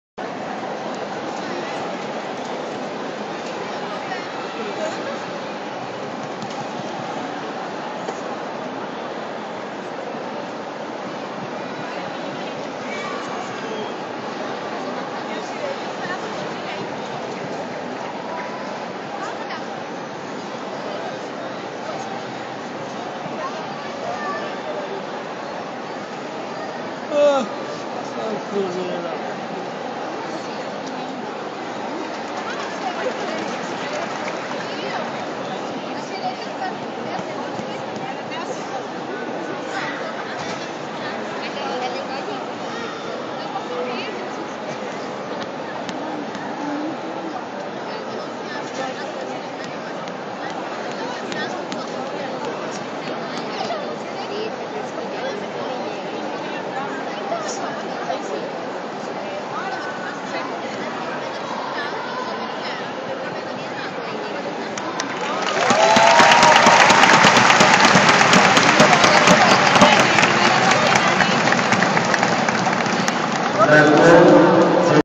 Heo Seon Mi (KOR) BB EF Meeting Internacional 2012
Presentation of Korean Seon Heo mi on balance beam at the international meeting of artistic gymnastics in Sao Bernado do Campo 2012.